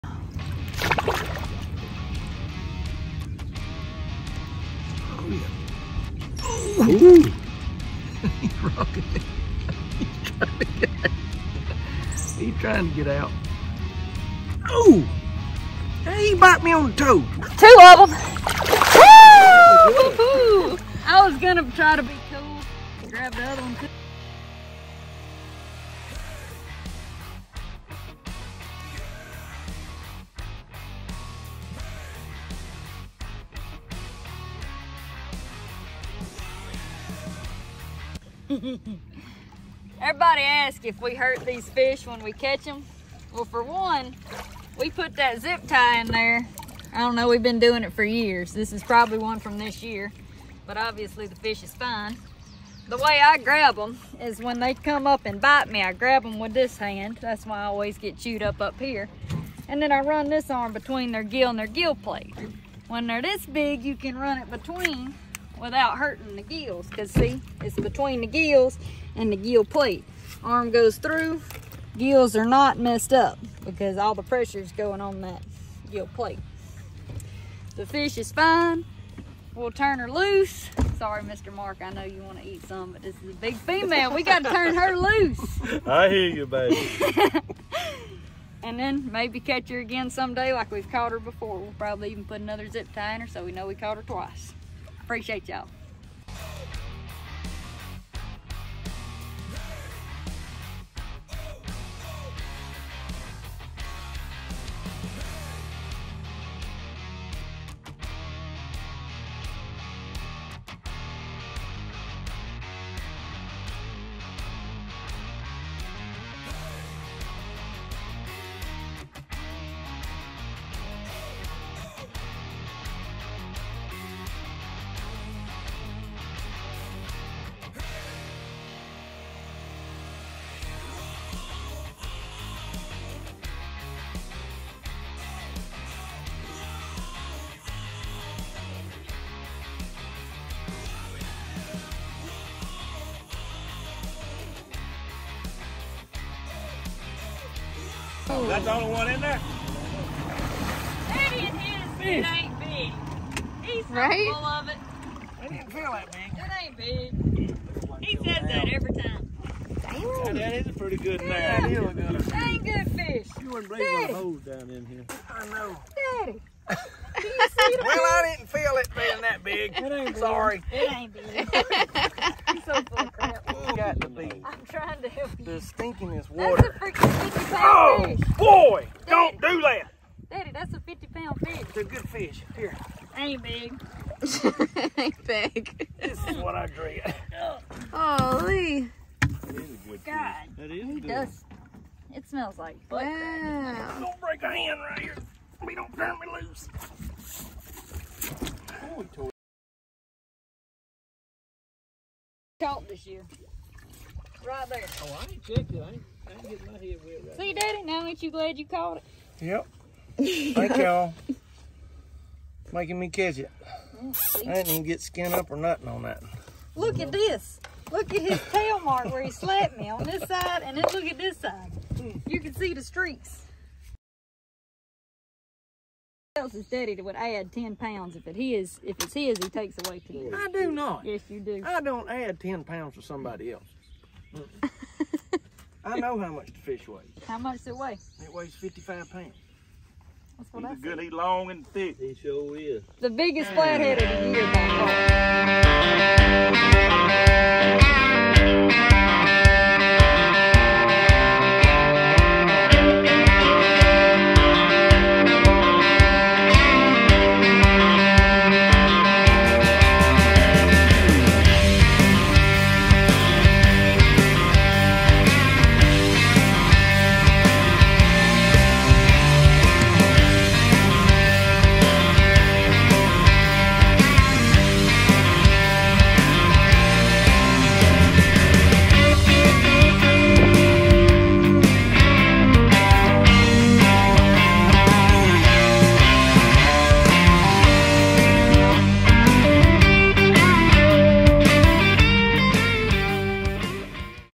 CATFISH NOODLING: Dang Good Day of Fishing! Dad caught the biggest Flathead of the year!
Preciate y’all!